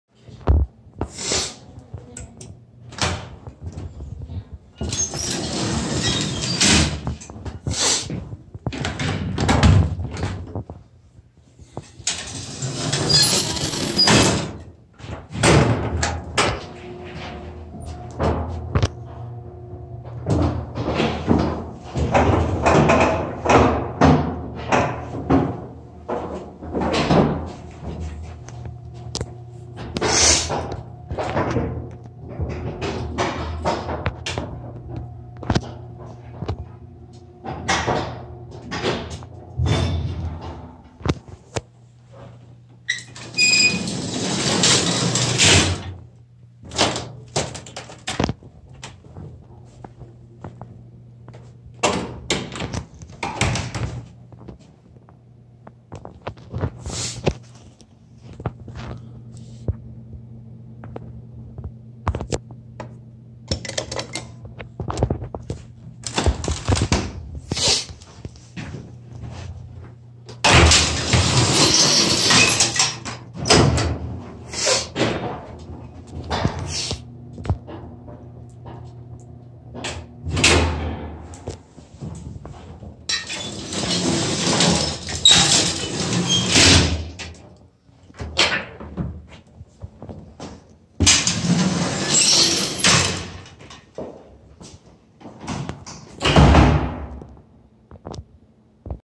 Old 'Gated' Turnbull Lift at Westminster Plaza Apartments, Halifax
Was introduced to this gem by my wife's sister and her boyfriend. This lift is currently inspected and still in great shape. It's seen slight upgrades throughout the years but it's still going strong. I didn't expect to find something like this in Halifax!